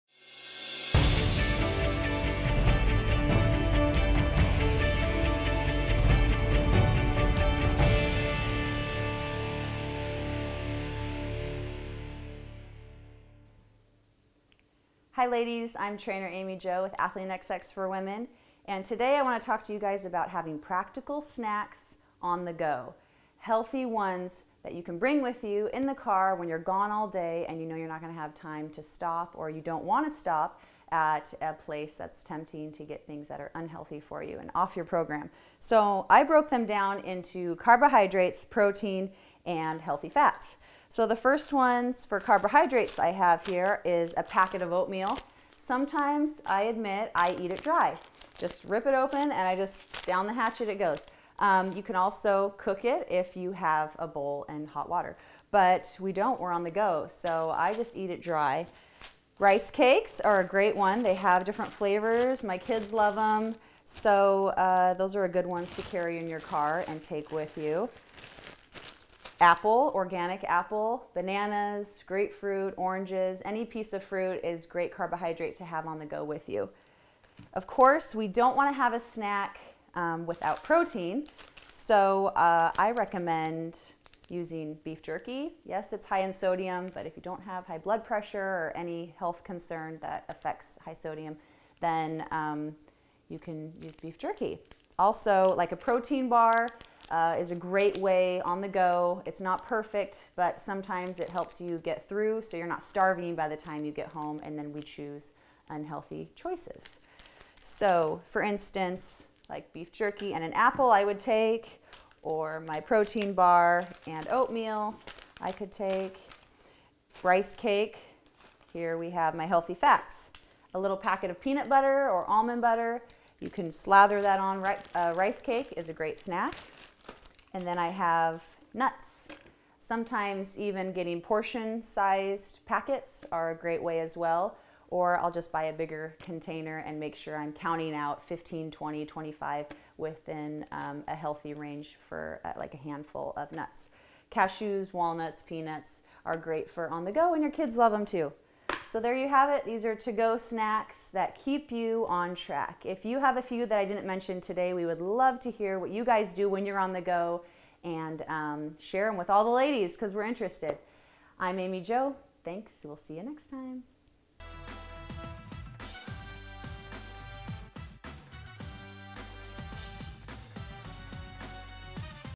Best High Protein Snacks for Women (MINI MEALS ON THE GO!!)
Best high protein snacks for women can be found in our 90 day fitness and nutrition system

The best high protein snacks for women are mini meals that you can eat on the go.  These high protein foods should be combined with healthy fats and carbs to make sure your diet is balanced and to keep you feeling satiated.  So today we’re sharing with you some of the best snacks for women that you can also take with you on the road.

These healthy low calorie snacks are easy to pack in your purse, gym bag or to leave in the glove compartment.  These are some of the best protein sources for women that are also totally portable.

Below are the best high protein snacks for women.  These make the perfect complement to a high protein diet plan which will be ideal to help women build muscle and get lean.

1) Packet of Oatmeal
2) Rice Cakes
3) Fruit
4) Beef Jerky
5) Protein Bar
6) Peanut or Almond Butter